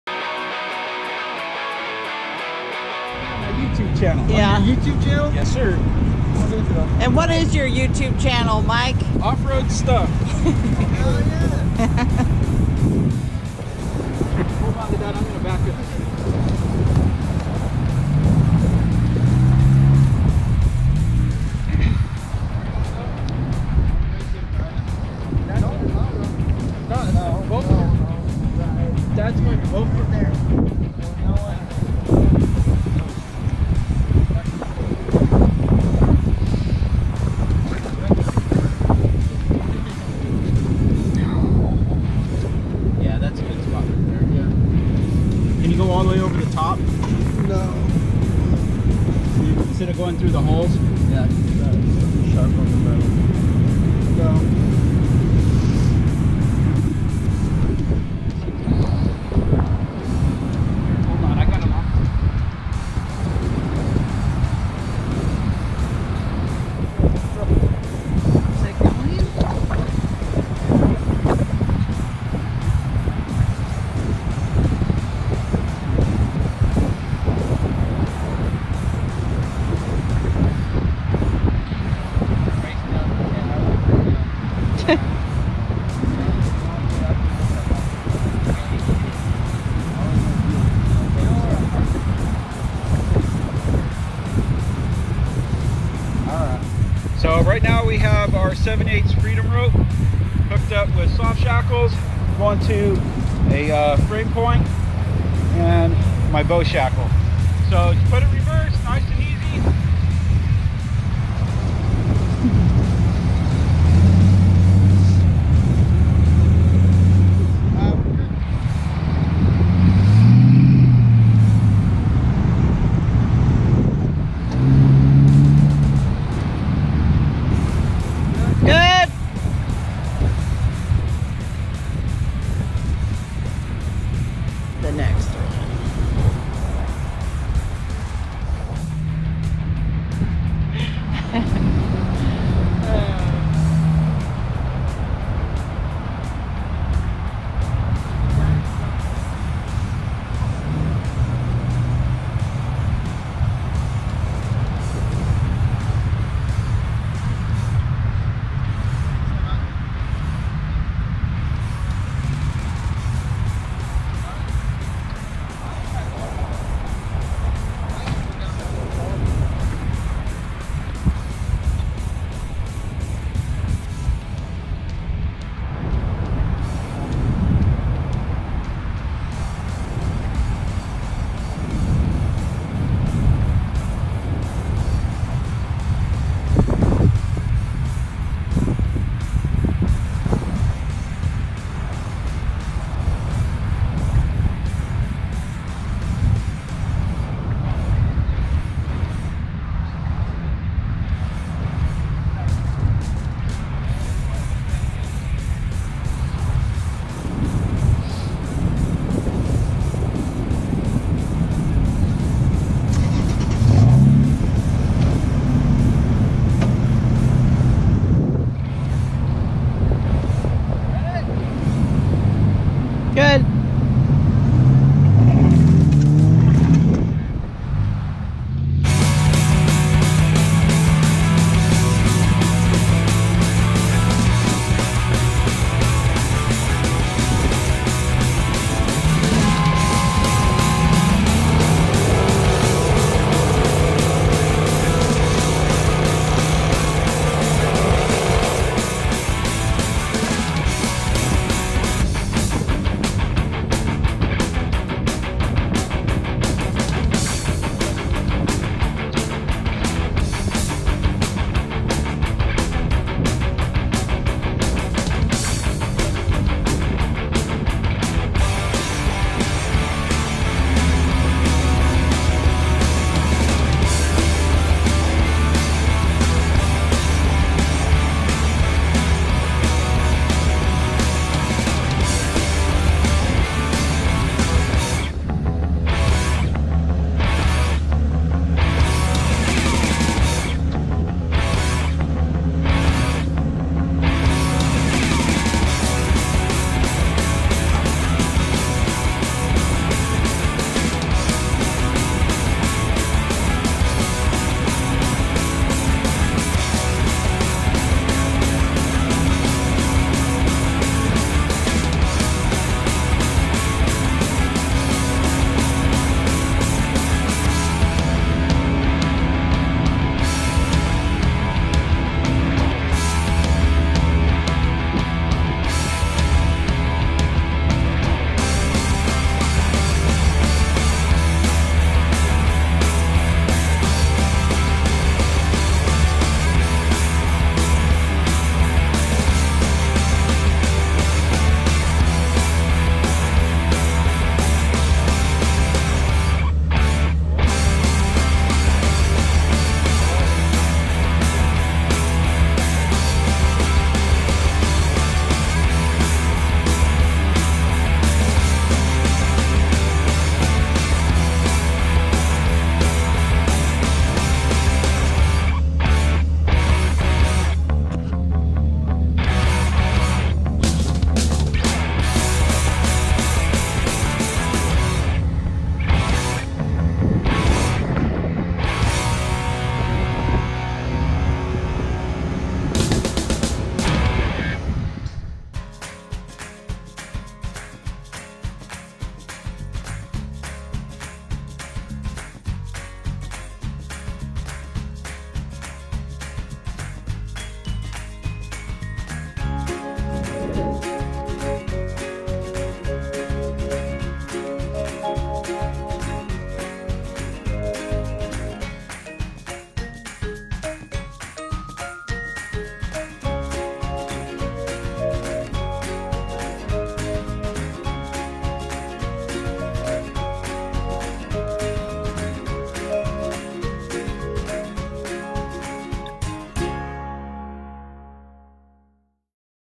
Stuck at the beach
Putting the Freedom Ropes to the test